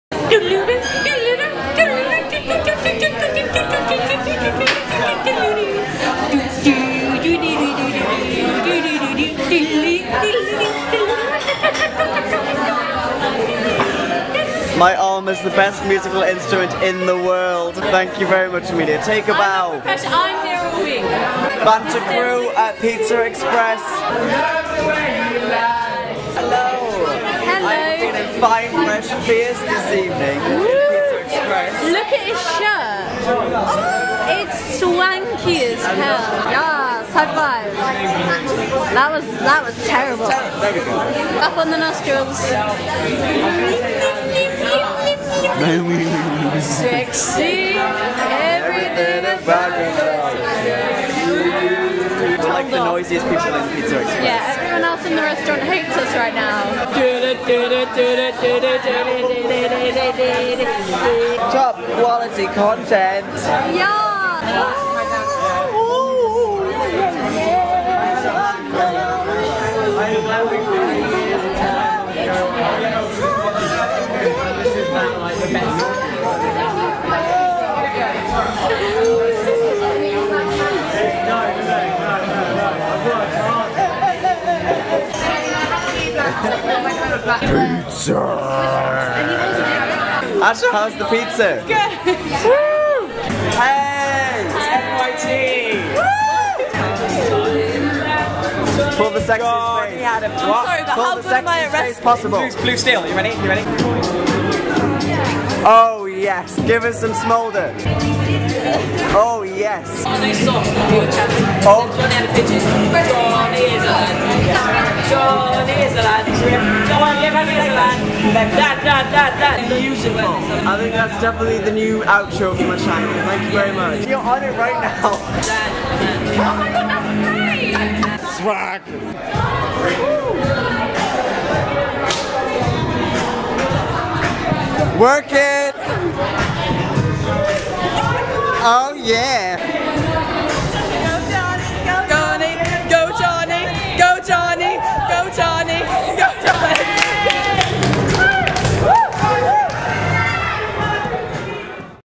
NATIONAL YOUTH THEATRE LARK
Little clips from other days with the wonderful NYT lot.